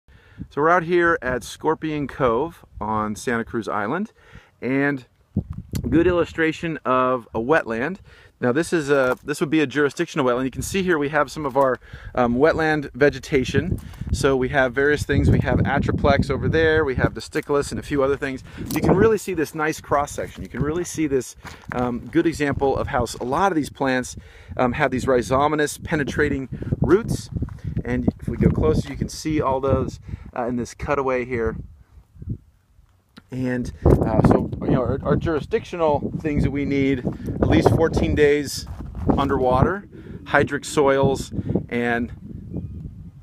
Scorpion Cove Wetland 2016
A quick look (before my memory filled) of a jurisdictional wetland at the terminus of Scorpion Creek on Santa Cruz Island, California.  This was recorded in mid September, 2016.